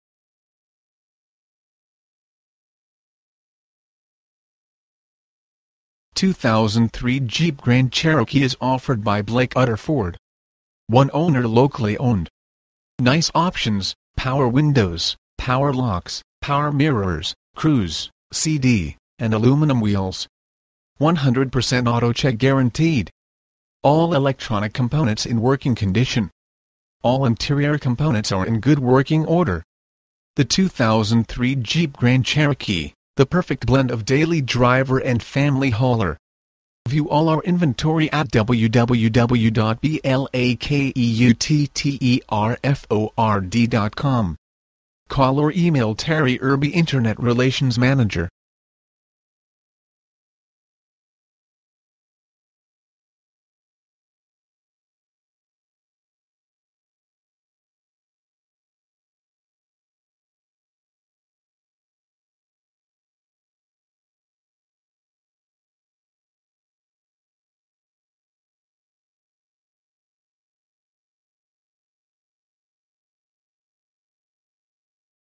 2003 Jeep Grand Cherokee - Bob Utter Ford Lincoln Kia - Sherman, TX 75090
Bob Utter Ford Lincoln Kia
2525 Texoma Pkwy

For Sale: 2003 Jeep Grand Cherokee
VIN: 1J4GX48S63C619142
Engine: 4.0L (242) SMFI I6 Power Tech engine
Drivetrain:
Transmission: Automatic
Mileage: 124,111
Color: Stone White (exterior) Sandstone (interior)

Features:
Two Thousand Three Jeep Grand Cherokee is offered by Blake Utter Ford.  One owner locally owned! Nice options: Power windows, power locks, power mirrors, cruise, cd, and aluminum wheels.  100% AutoCheck guaranteed! All electronic components in working condition. All interior components are in good working order. The Two Thousand Three Jeep Grand Cherokee: the perfect blend of daily driver and family hauler.  . Call or email Terri Irby Internet Relations Manager.